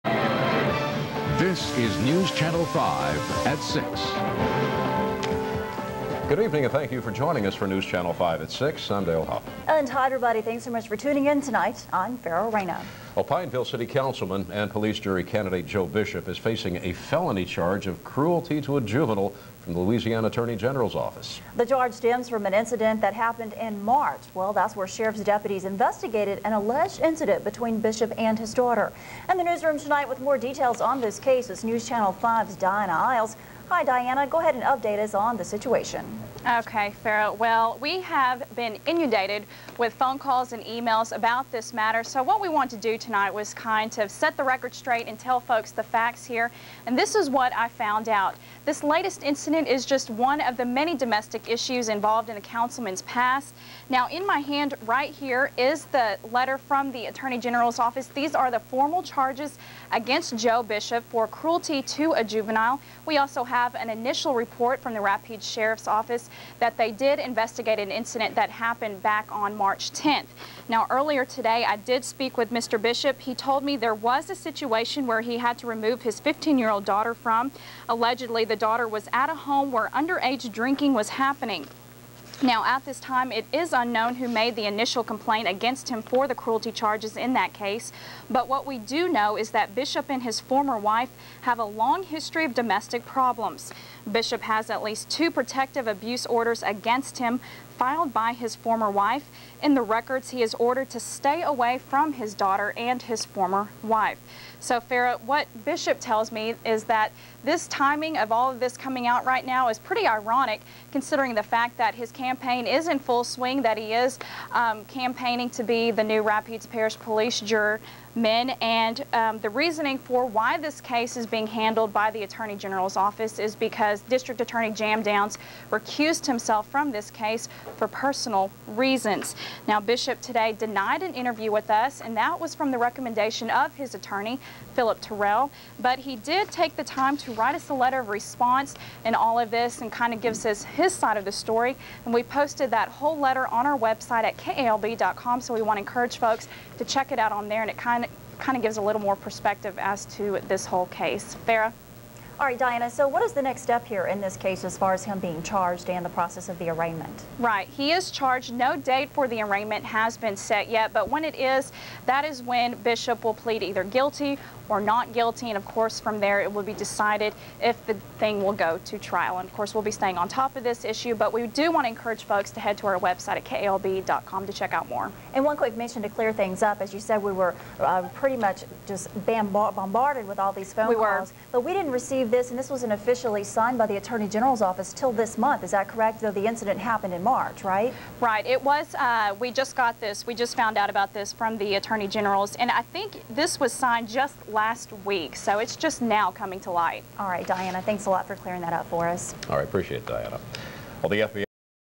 Pineville Councilman Joe Bishop Arrest Report 6:00 pm
news report of pineville city councilman joe bishop arrest.